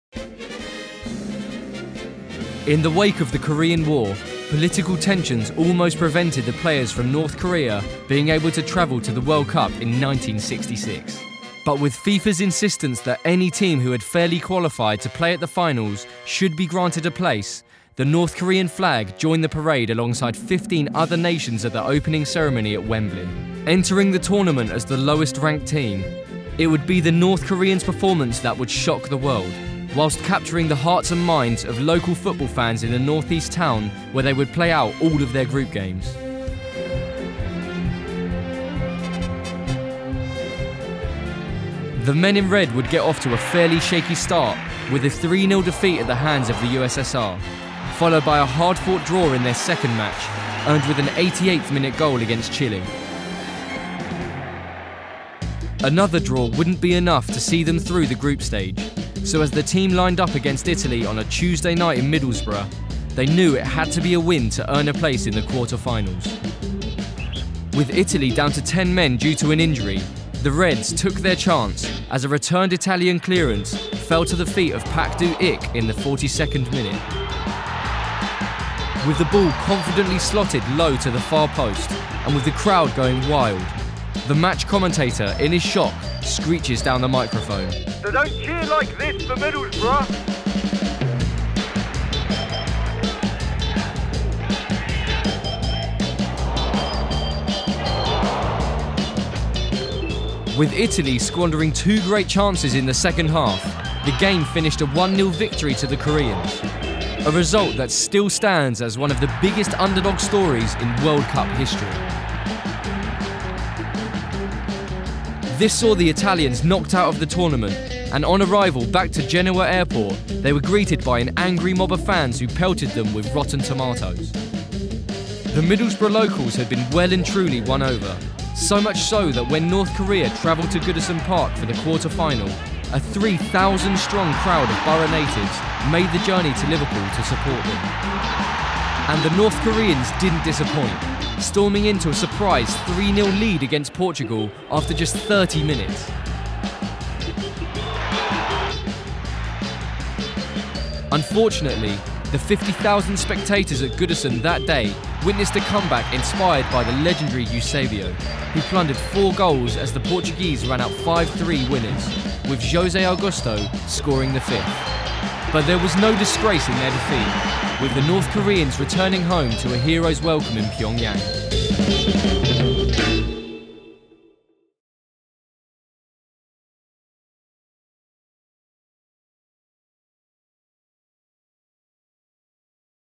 When Middlesbrough went North Korean | 1966 World Cup
At the 1966 World Cup, North Korea found an unlikely ally in the North East of England, an ally which helped see them defy all odds to pull off one of the greatest upsets in football history.